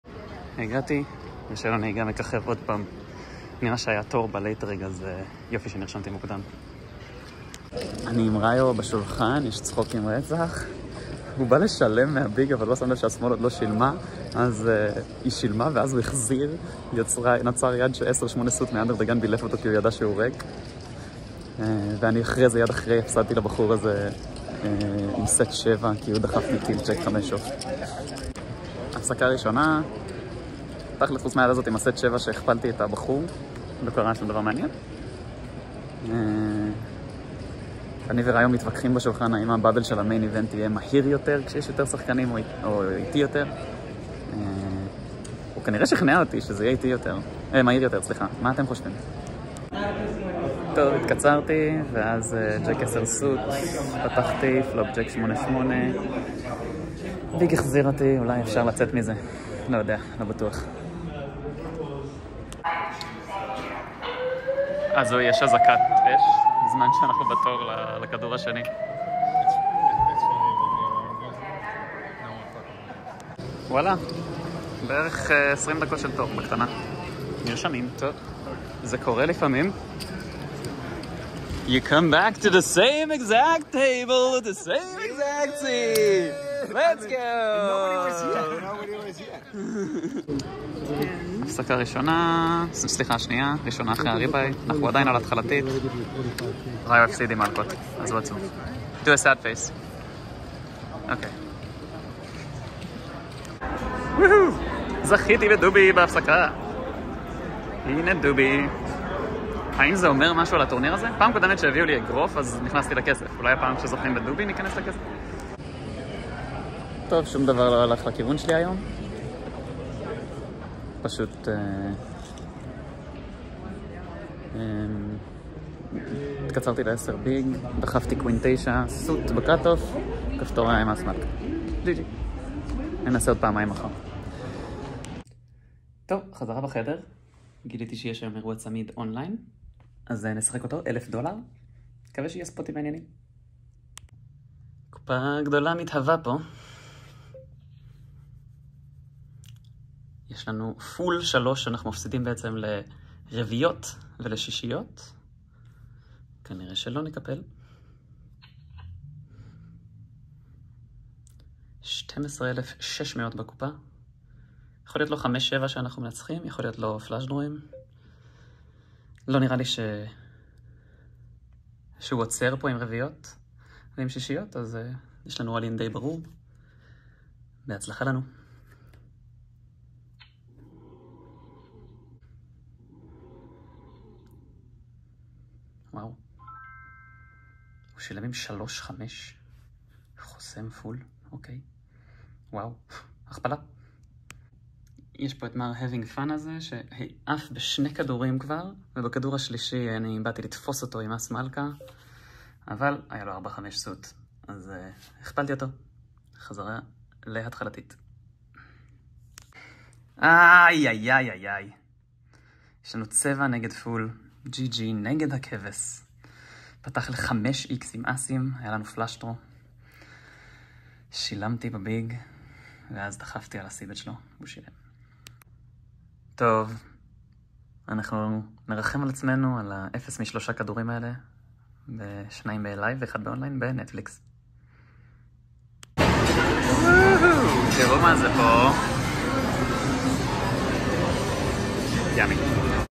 Alon’s WSOP 2023 vlog, Day #9!
777$ 7מקס.
טורניר אונליין 1000$
האם מצאתי את הקש הרביעי שלי באליפות?